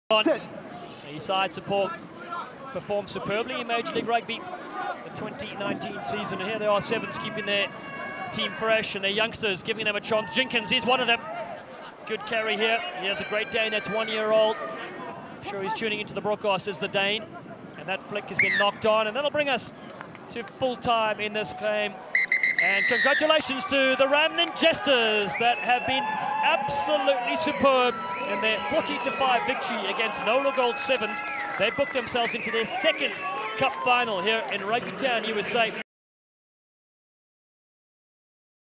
Final Play Of The Game: NOLA GOLD v RAMBLIN JESTERS RugbyTown 7s Cup Semifinal 2019
FINAL MOVEMENT OF THE MATCH KNOCK ON FULL TIME